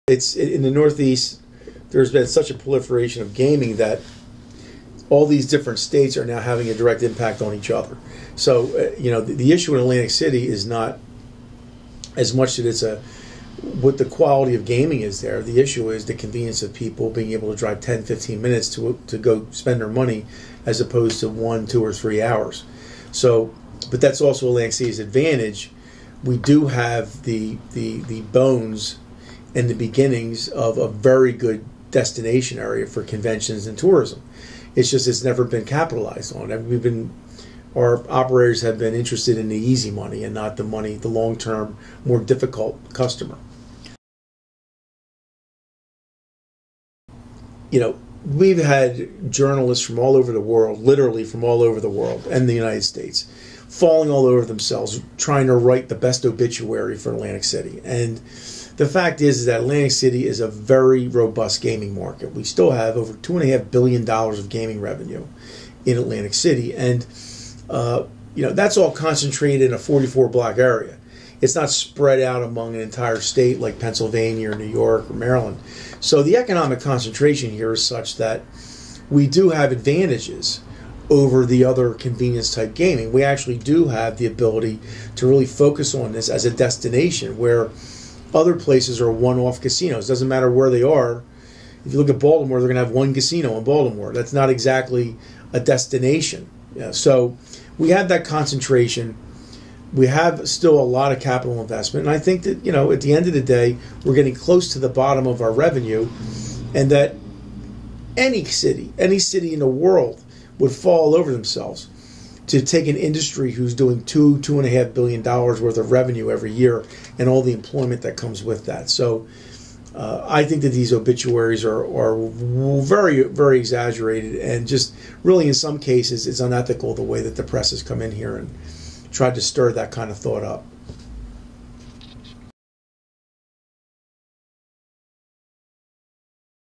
Union leader Bob McDevitt talks about the pluses of Atlantic City
Union leader Bob McDevitt talks about Atlantic City and the rumors of its demise in the popular press. (Kevin G. Hall/McClatchy)